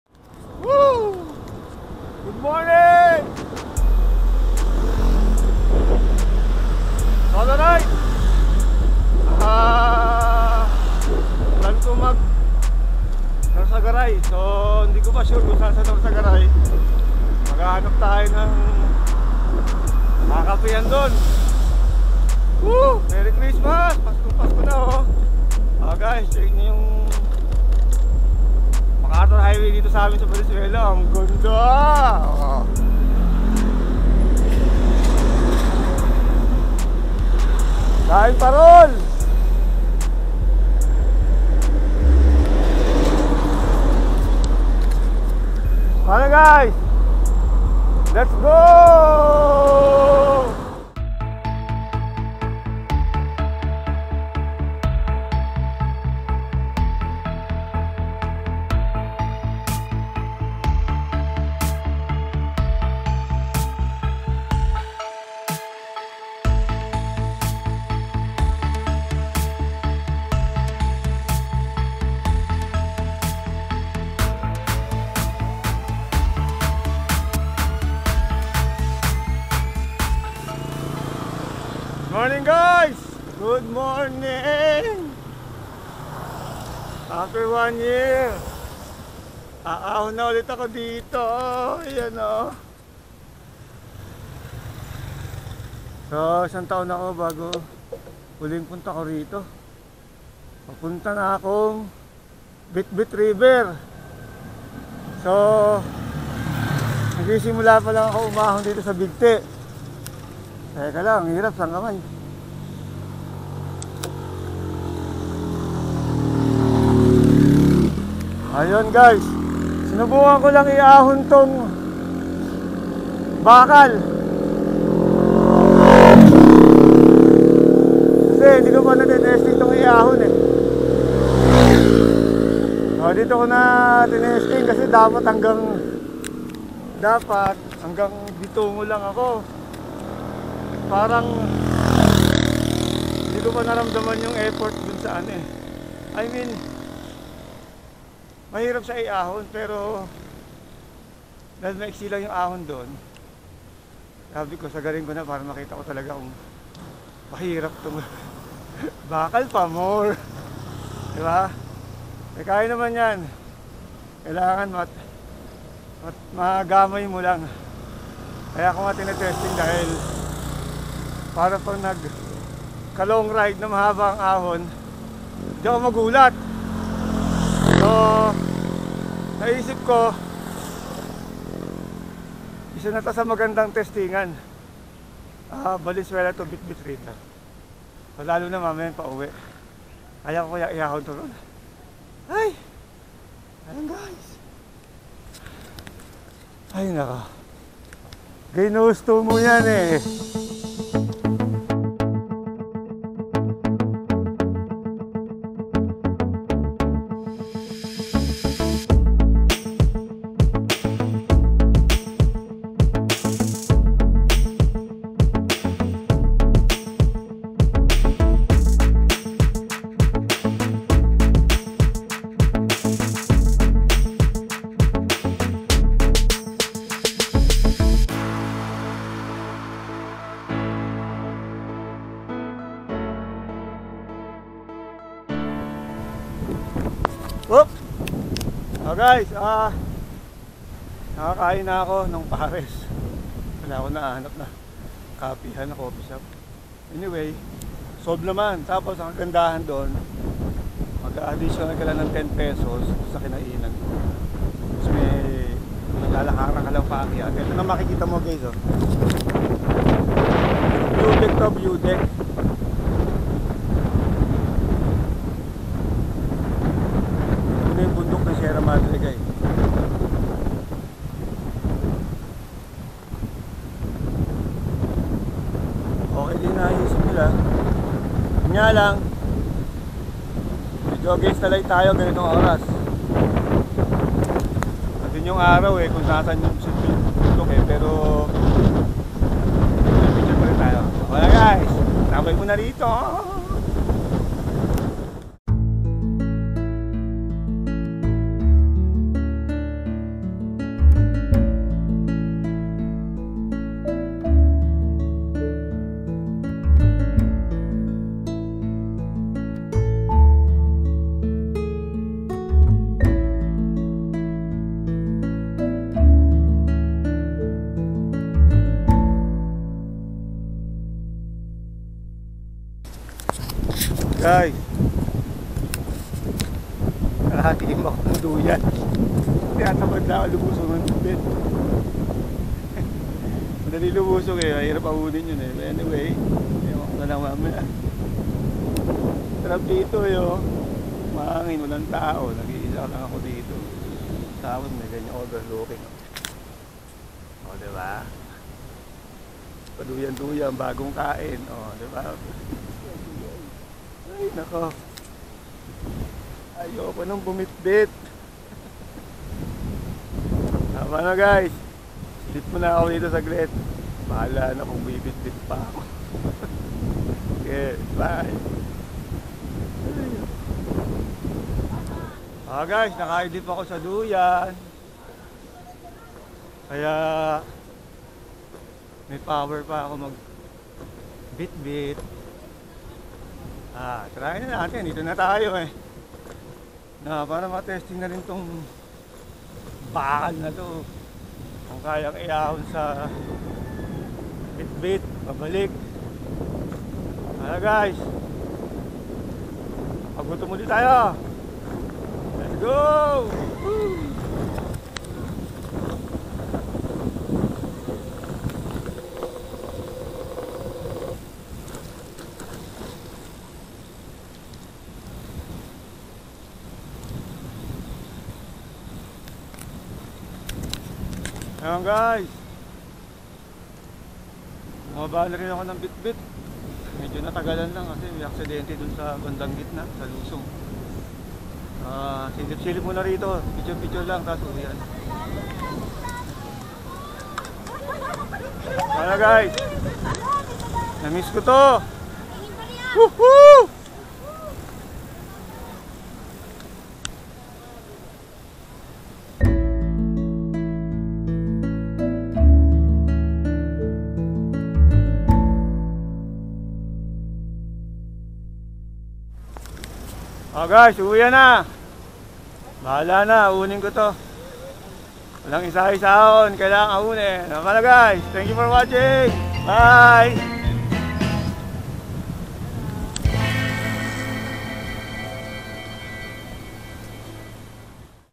Pilit Na Pilit Sa Bitbit 😅 | Steel is Real
A quick solo ride going to Bitbit Bridge in Norzagaray Bulacan just to test my newly build Ave Maldea steel touring bike.